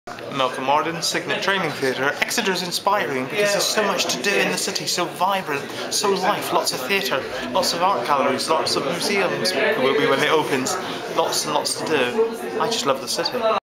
Exeter Macolm Mardon.AVI
Malcolm Mardon, Asst Administrator, Cygnet Training Theatre talking about what interests and excites him about Exeter.

Talking to Michael Noonan, at Exeter Cultural Partnership meeting 13th July 2011 Reed Hall, Exeter University.